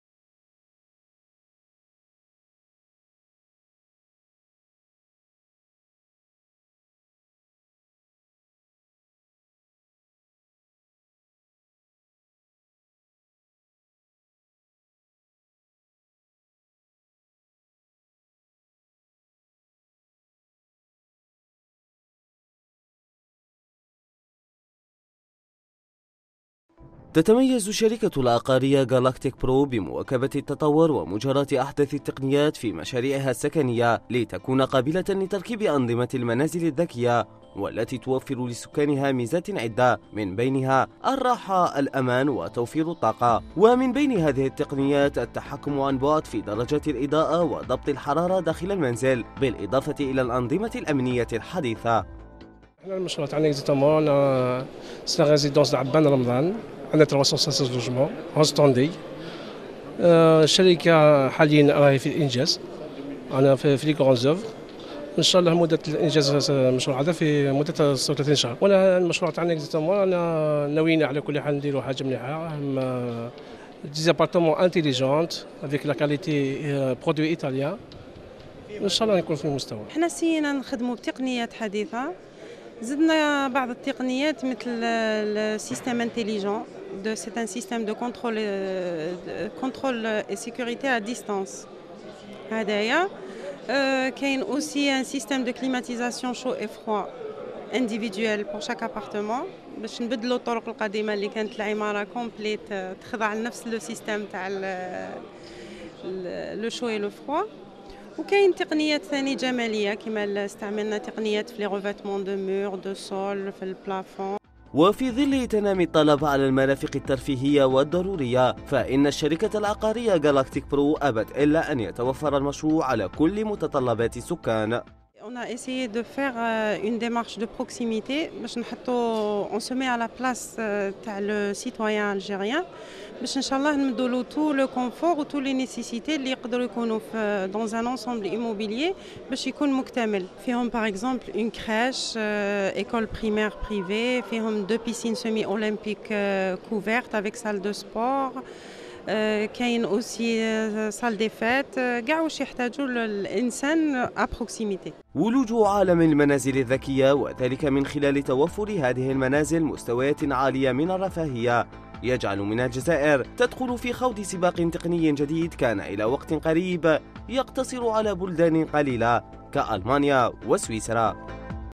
GalacticPro Residance Abane Ramdane dzair news
Projet de réalisation « Résidence Abbane Ramdane 316 logts Kolea

- Des appartements vous sont proposé de type f3 de : 111M² et 113M², f4 de : 146M²et 150M², f5 en duplex de 375M² et 383M² dans une résidence clôturée et gardée située au sud-est de la ville de kolea et du pôle universitaire

Situation de la résidence :
 - Proche d’Alger, Blida et Tipaza (15/20 minute par véhicule)
- Loin d'embouteillage , pas loin de la mer avec une belle vue sur espace verts naturel
- A proximité d'équipement collectifs projetés : primaire , lycée et polyclinique
- Constituée de 11 blocs de: R+7 à R+9 , 4 à 2 appartement par palier avec deux ascenseurs à chaque bloc accessible à partir du parking
- Bénéficie des espaces verts et air de jeux , piscine couverte , salle de sport , salle des fêtes  école privée , crèche , des surfaces commerciale et services
- La Gestion de la résidence sera assuré par le promoteur pendant 2 an en prévision d’une structure tiers
-Parking en deux sous-sols avec box aussi

Pour plus d'information visitez notre site web: galacticpro.com